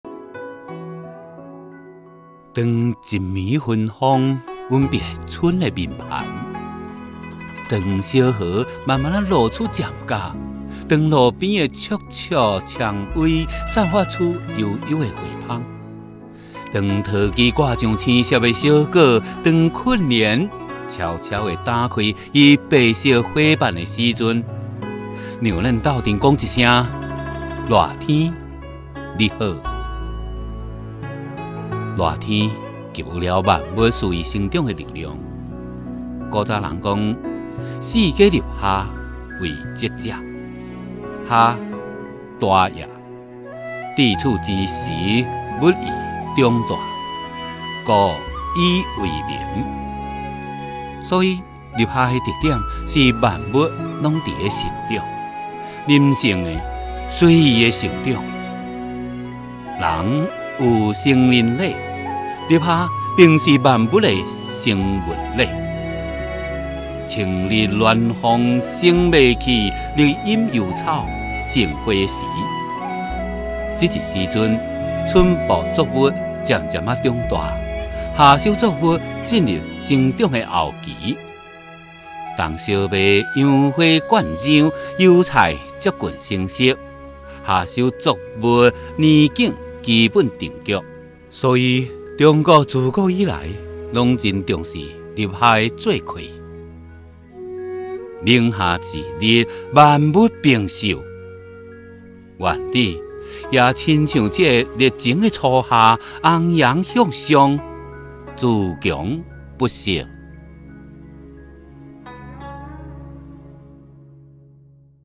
方言廿四节气：立夏∣闽南话
方言廿四节气 夏天 你好
立夏，被视为夏天的开始。当一夜熏风吻别春的脸庞，当小荷慢慢露出尖角，当路边的簇簇蔷薇散出幽幽花香，当桃枝挂上青涩的小果，让我们一起说一声：夏天，你好！